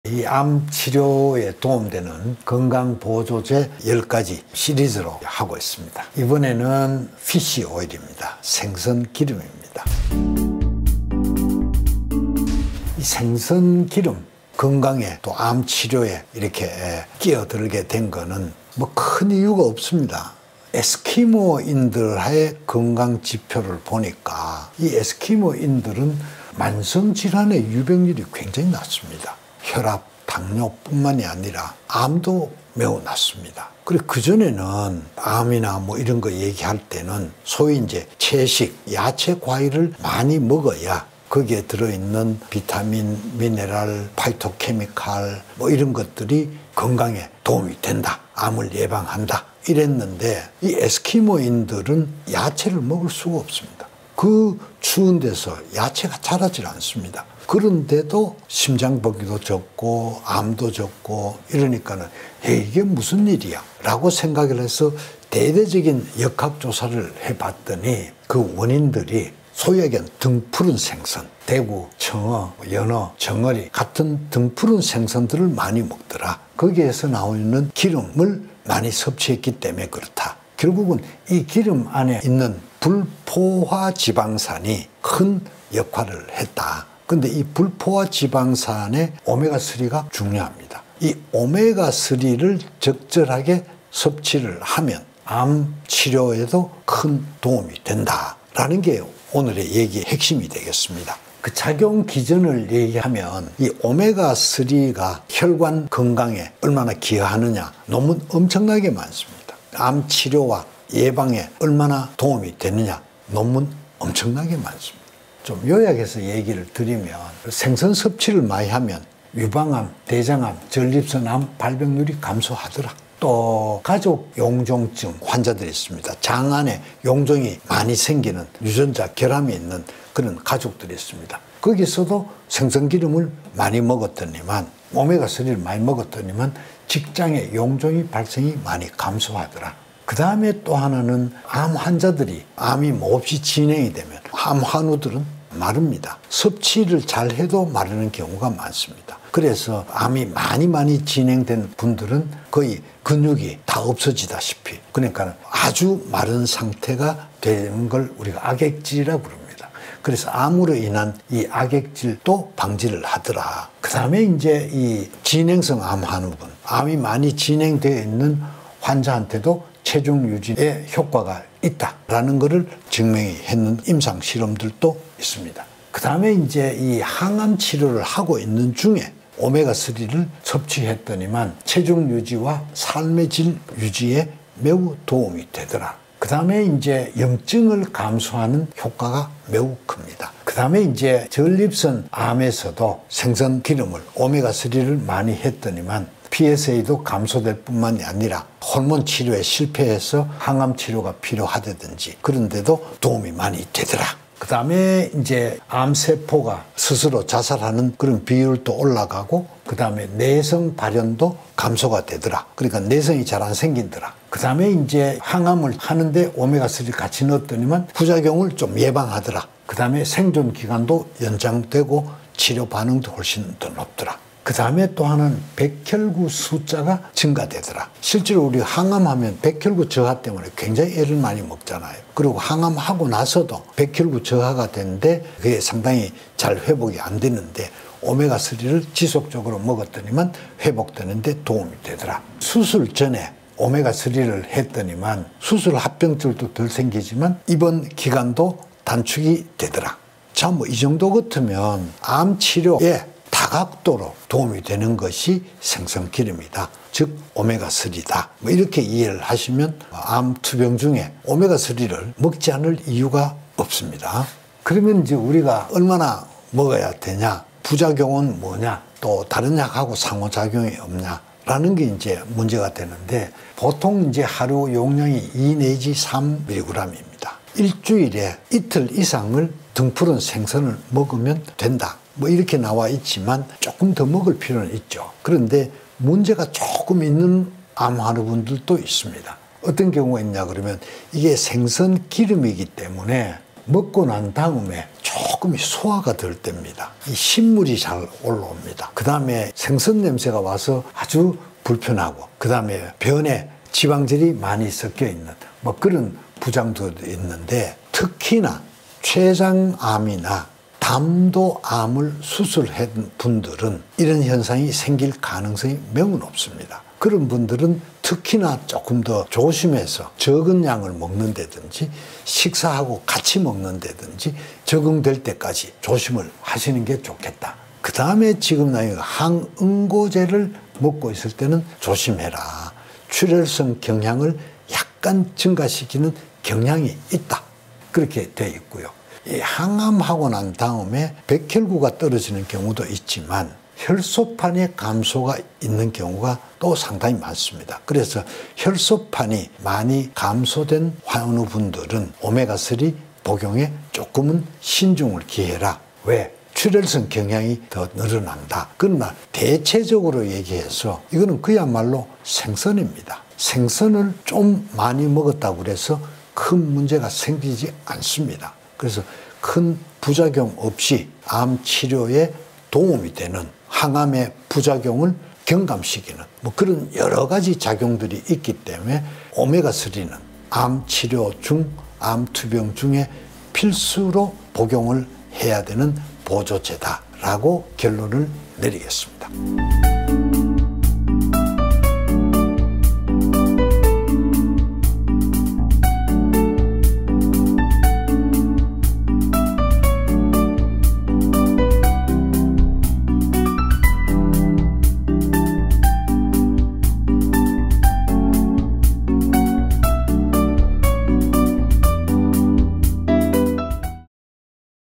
암 치료에 도움되는 건강보조제 피쉬 오일, 오메가3
암 환자에게 필수적인 건강보조제, 오메가-3
이 오메가-3를 적절하게 섭취하면 암 치료에 큰 도움이 된다는 게 오늘 얘기의 핵심이 되겠습니다.

암 치료에도 전략이 필요합니다. 적절한 치료법의 선택과 본인의 투병 의지, 투병 방식과 같은 자기 자연치유능력을 향상하게 하기 위한 노력이 필요합니다. 단순히 항암제만 맞는 것이 아니라 항암치료를 전략적으로 이용해서 여러가지 방법을 동원해야만 효율적인 암 치료가 이루어질 수 있습니다.